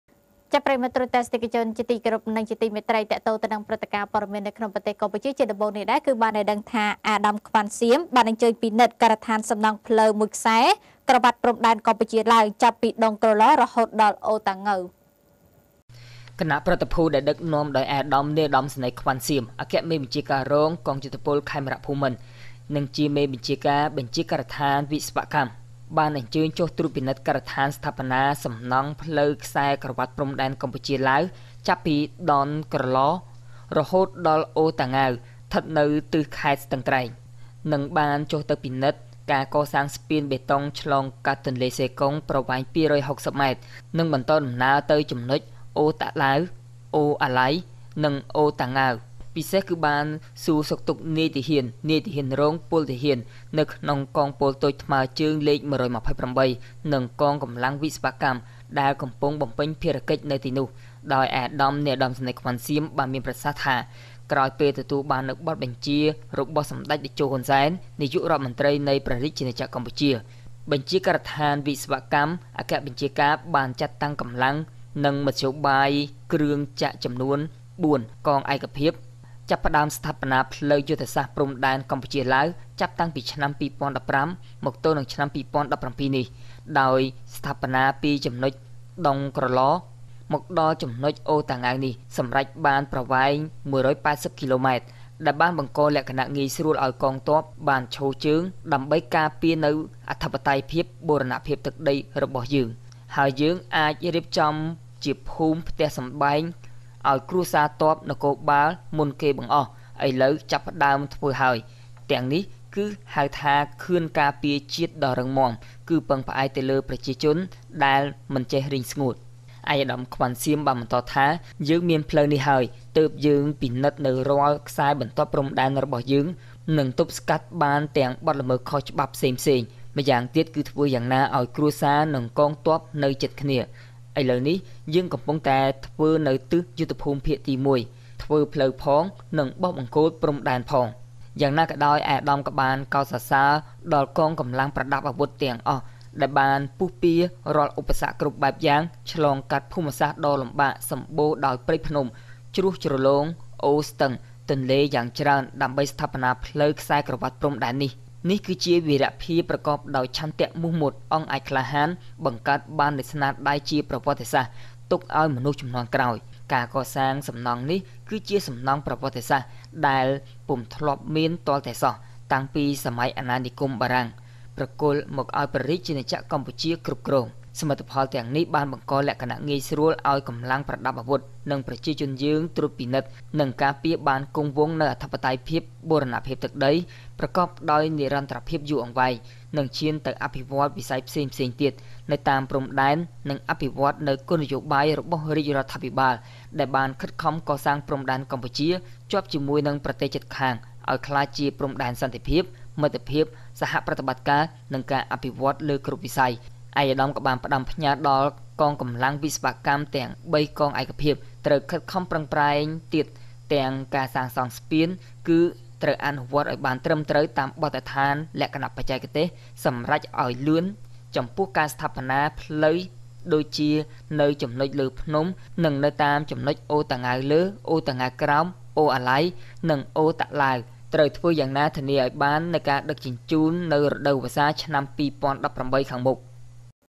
ឯកឧត្តមខ្វាន់ស៊ាមអញ្ជើញពិនិត្យការដ្ឋានសំណង់ផ្លូវខ្សែក្រវ៉ាត់ព្រំដែនកម្ពុជា-ឡាវចាប់ពីដុងក្រឡ
ឯកឧត្តមខ្វាន់ស៊ាមអញ្ជើញពិនិត្យការដ្ឋានសំណង់ផ្លូវខ្សែក្រវ៉ាត់ព្រំដែនកម្ពុជា-ឡាវចាប់ពីដុងក្រឡ រហូតដល់អូរតាង៉ាវ